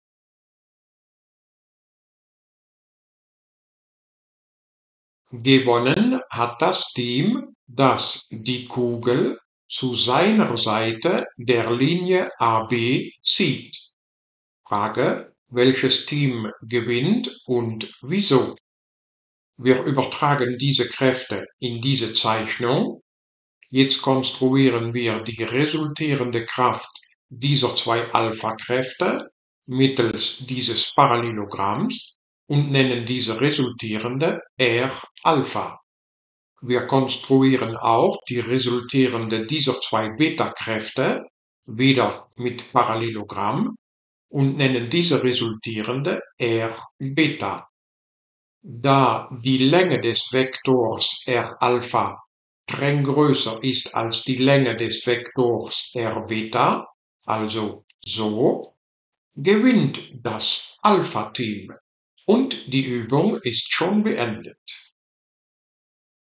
Physik 4PhD048 Kraftvektoren
Kräfte sind Vektoren und müssen also vektoriell addiert werden.